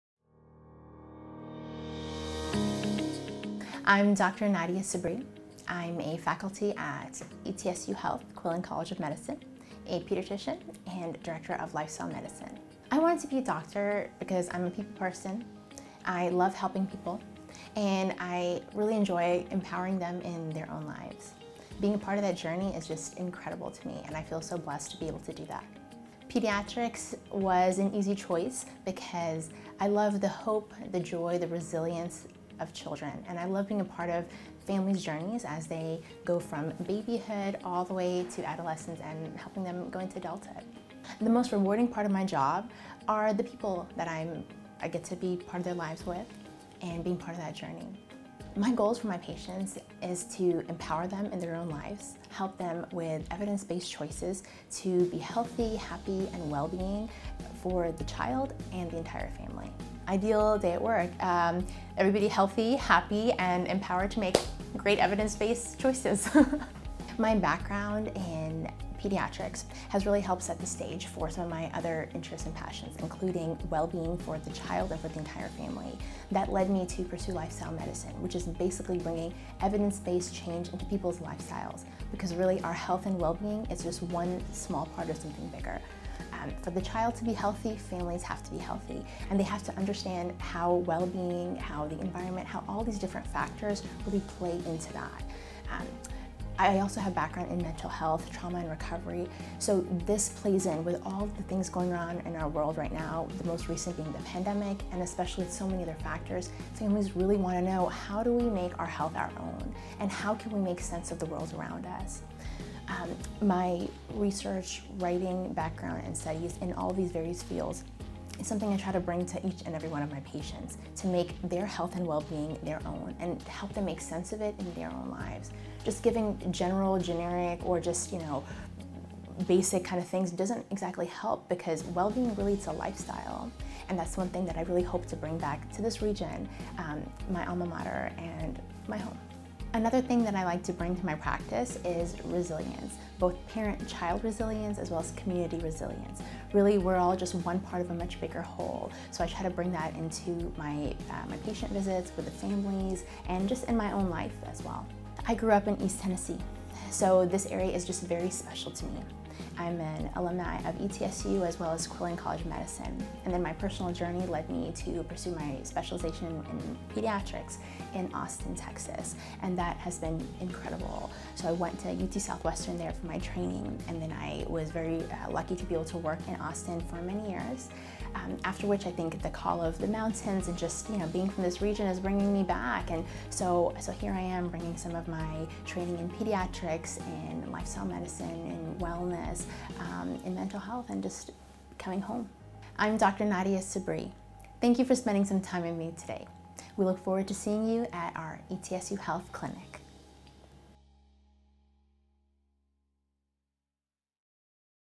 Dr. Nadia Sabri Interview
Dr. Nadia Sabri is a pediatrician at ETSU Health Pediatrics Johnson City.